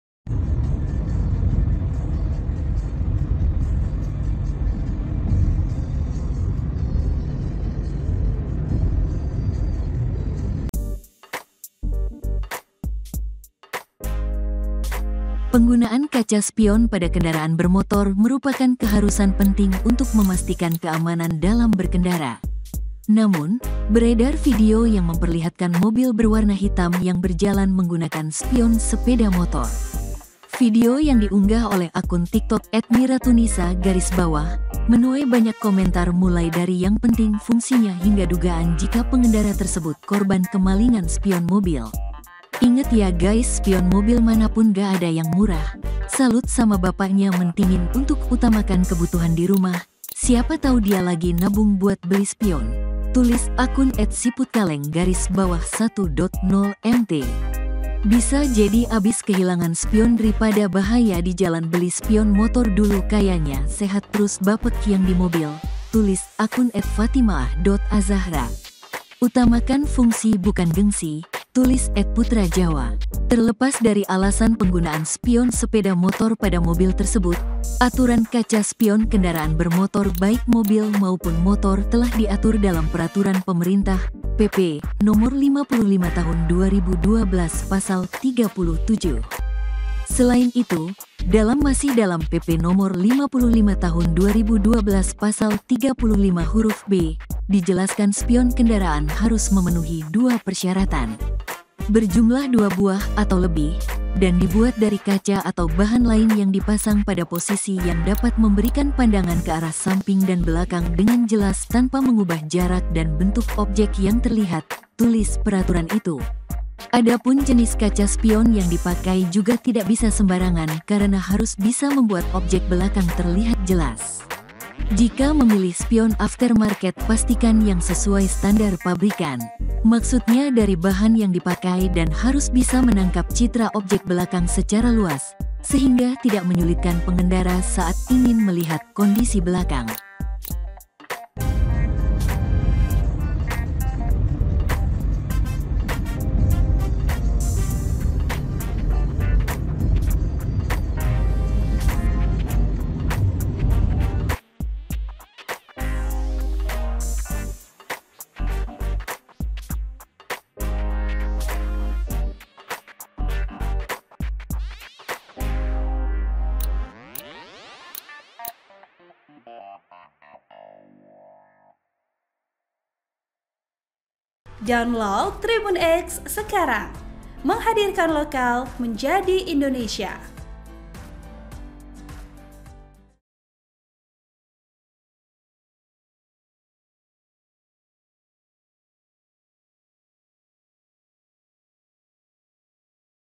Mobil Pakai Spion Sepeda Motor
Penggunaan kaca spion pada kendaraan bermotor merupakan keharusan penting untuk memastikan keamanan dalam berkendara. Namun, beredar video yang memperlihatkan mobil berwarna hitam yang berjalan menggunakan spion sepeda motor.

Video yang diunggah oleh akun TikTok @miratunisaa_, menuai banyak komentar mulai dari yang penting fungsinya hingga dugaan jika pengendara tersebut korban kemalingan spion mobil.

“Inget ya gais spion mobil manapun gaada yg murah, salut sama bapaknya mentingin untuk utamakan kebutuhan dirumah, siapa tau dia lagi nabung buat beli spion,” tulis akun @siputkaleng_1.0mt.

“Bisa jdi habis kehilangan spion dripada bahaya di jalan beli spion motor dulu kayanya sehat trus bapk yg di mobil,” tulis akun @faatimaah.azzahraa.

“Utamakan fungsi bukan gengsi,” tulis @putra jawa.

Terlepas dari alasan penggunaan spion sepeda motor pada mobil tersebut, aturan kaca spion kendaraan bermotor baik mobil maupun motor telah diatur dalam Peraturan Pemerintah (PP) Nomor 55 Tahun 2012 Pasal 37.

Selain itu, dalam masih dalam PP Nomor 55 Tahun 2012 Pasal 35 huruf b, dijelaskan spion kendaraan harus memenuhi dua persyaratan.

“Berjumlah 2 buah atau lebih, dan dibuat dari kaca atau bahan lain yang dipasang pada posisi yang dapat memberikan pandangan ke arah samping dan belakang dengan jelas tanpa mengubah jarak dan bentuk objek yang terlihat," tulis peraturan itu.

Adapun jenis kaca spion yang dipakai juga tidak bisa sembarangan karena harus bisa membuat objek belakang terlihat jelas.

Jika memilih spion aftermarket pastikan yang sesuai standar pabrikan. Maksudnya dari bahan yang dipakai dan harus bisa menangkap citra objek belakang secara luas, sehingga tidak menyulitkan pengendara saat ingin melihat kondisi belakang.

Editor Video: Andri Idra
Uploader: